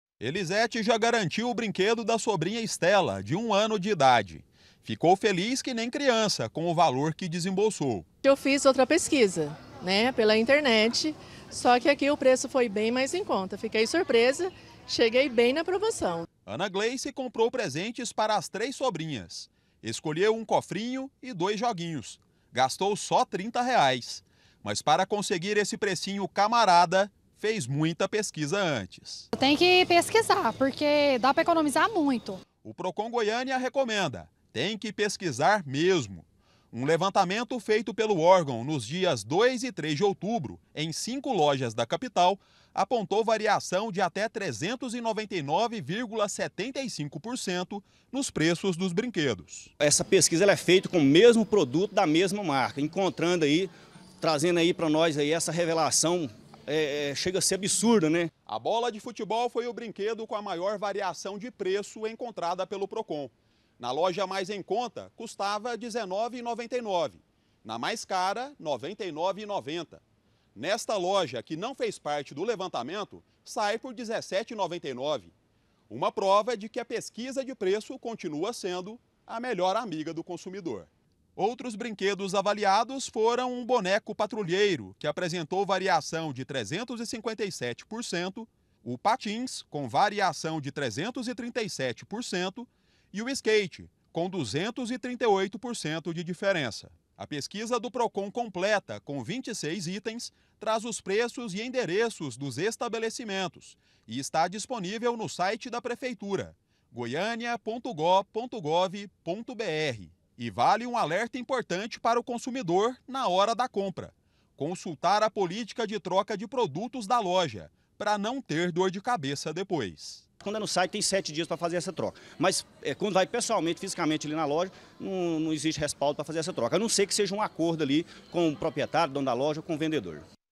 DIA DAS CRIANÇAS: PREÇOS DE BRINQUEDOS VARIAM ATÉ 400%
SE VOCÊ TEM CRIANÇA E AINDA AINDA NÃO COMPROU O PRESENTE PRA ELA, MELHOR PESQUISAR. O PROCON DIZ QUE A DIFERENÇA DE PREÇOS É GIGANTE.